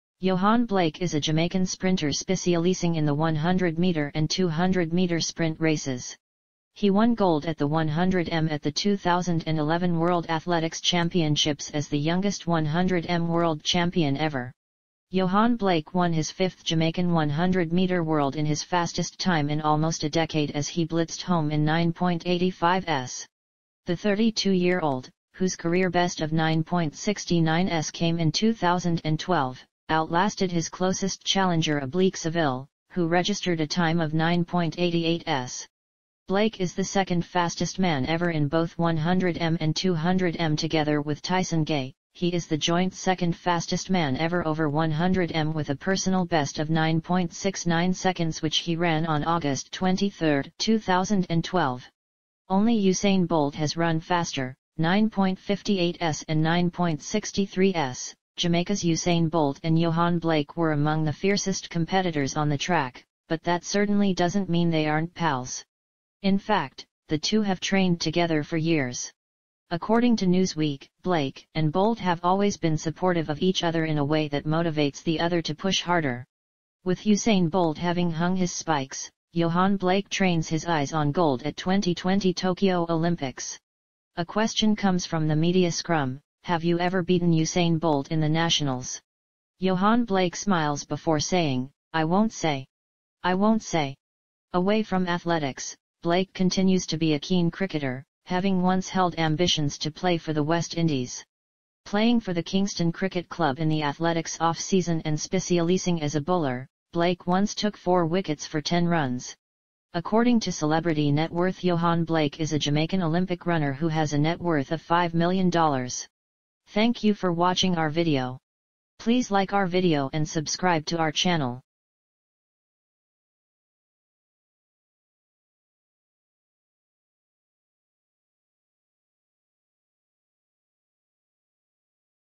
Yohan Blake Jamaican Sprinter Lifestyle & Net Worth 2022-He Is Going For Gold At The Olympics
Yohan Blake is a Jamaican sprinter specialising in the 100-metre and 200-metre sprint races. He won gold at the 100 m at the 2011 World Athletics Championships as the youngest 100 m world champion ever, Yohan Blake Jamaican sprinter Lifestyle & Net Worth#celebrities YohanBlake #athletics